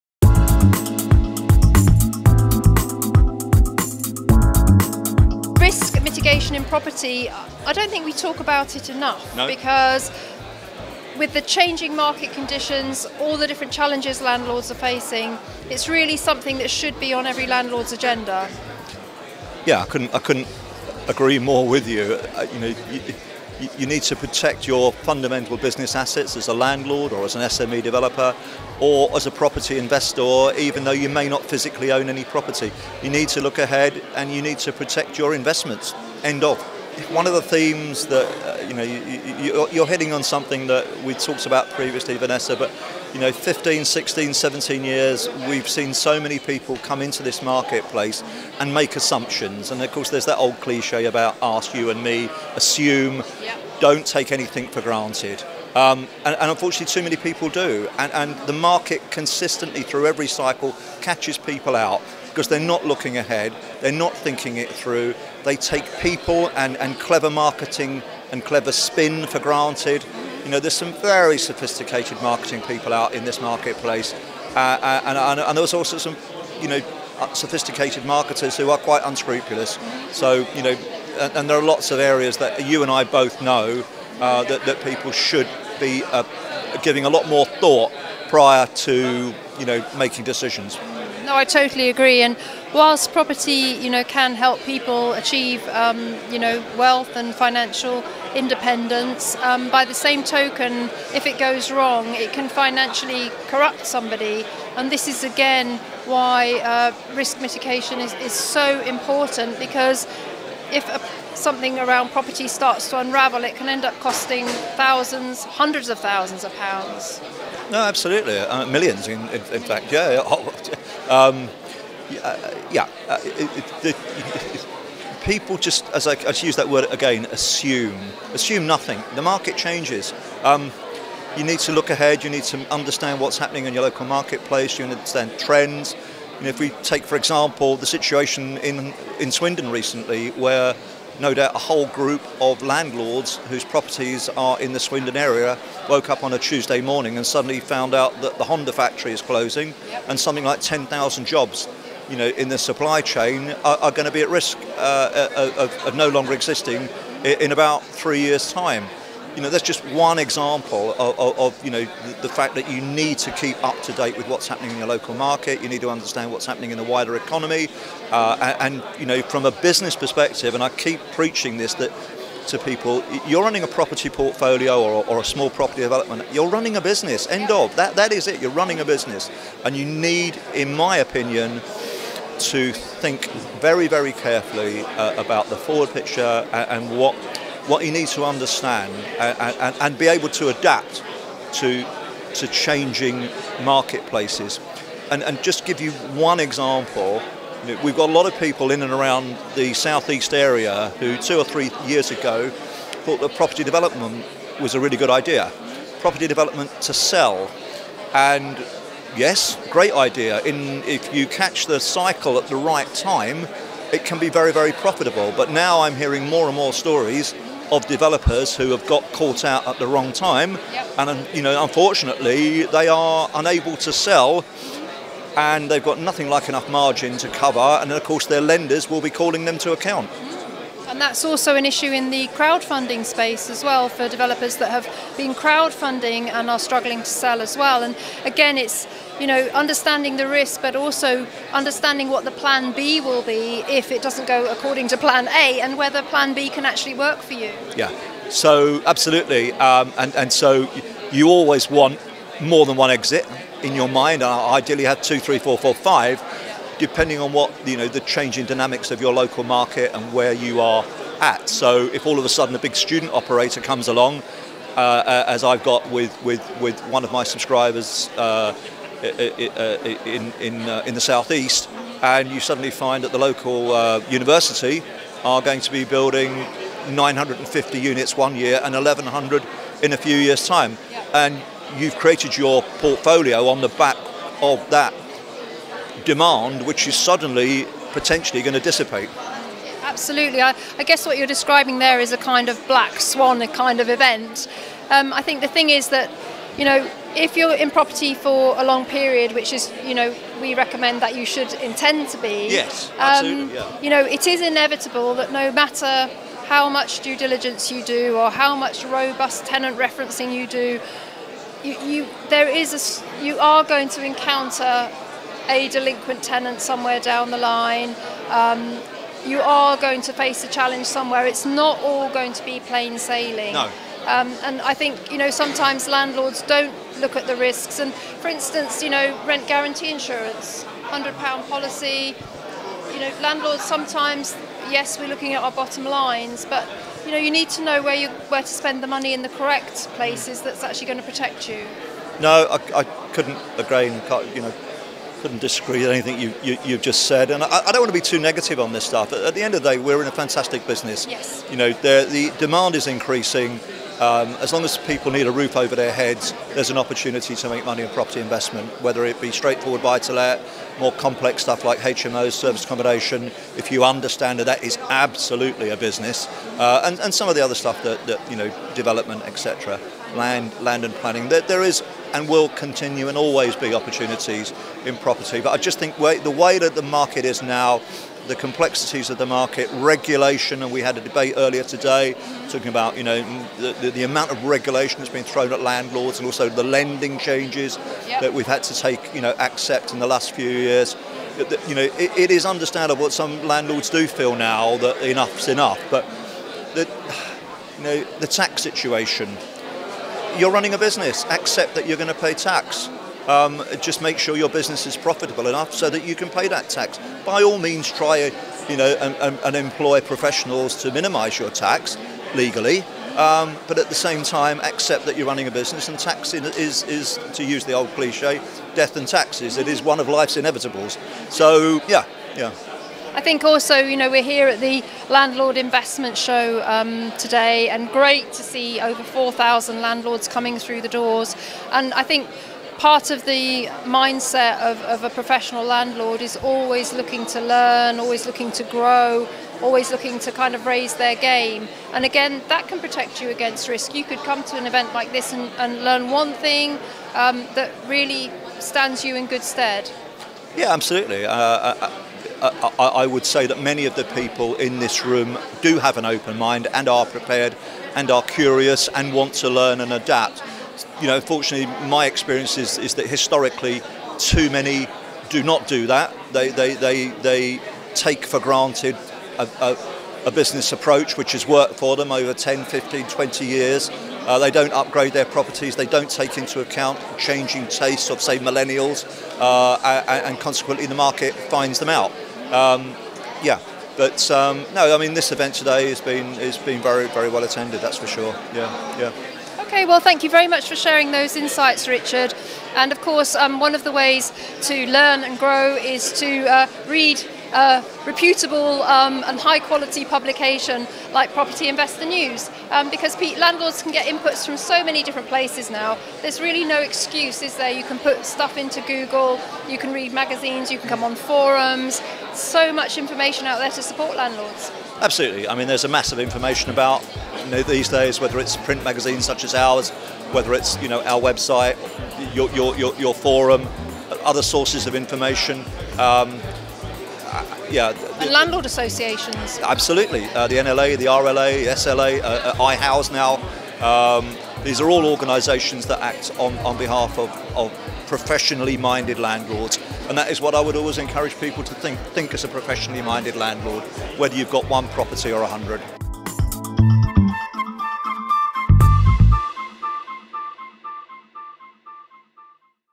Mitigating risk in property investment
Richard Bowser, Editor of Property Investor News, shares some insights in how landlords and property investors can mitigate risk of financial loss.